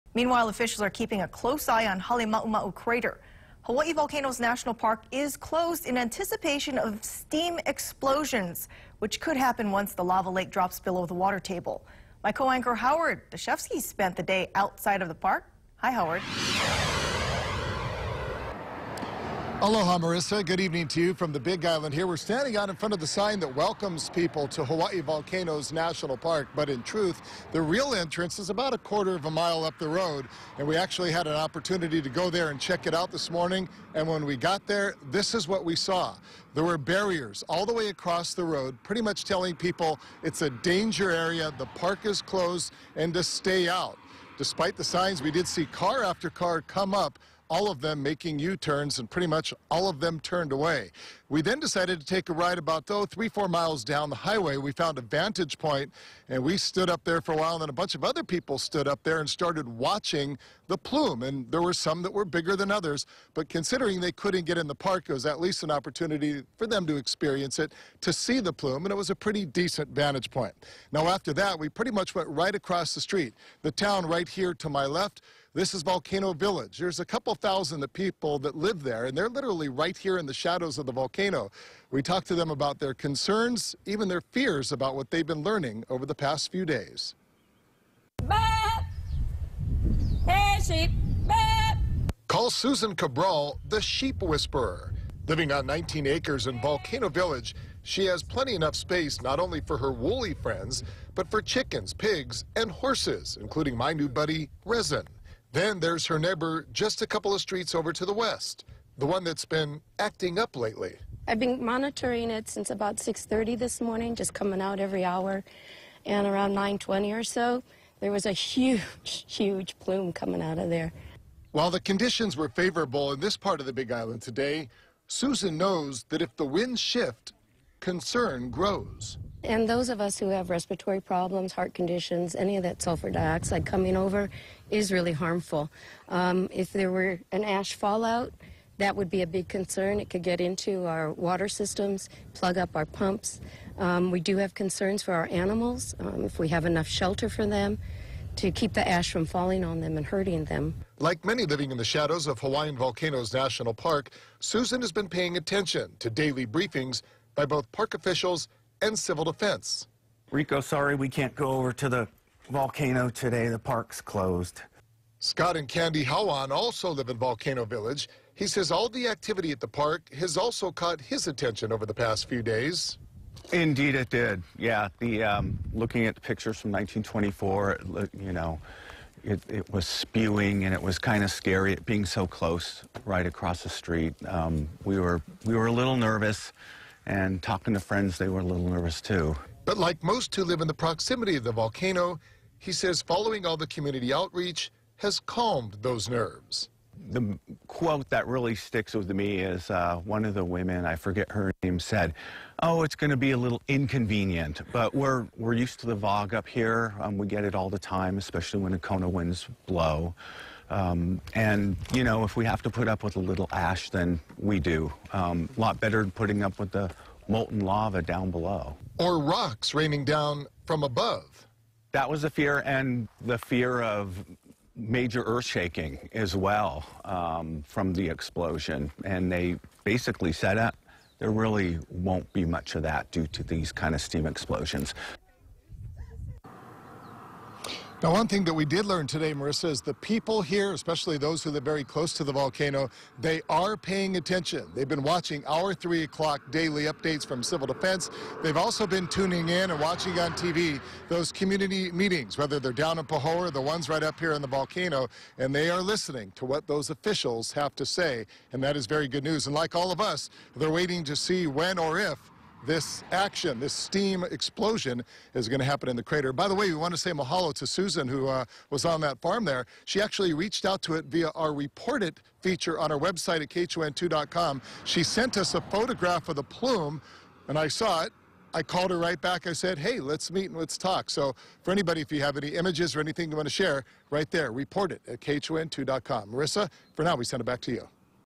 Residents living in Kilauea's shadow keep close eye on summit rumblings
"Looking at the pictures from 1924, it was crazy, and it's kind of scary being so close, right across the street."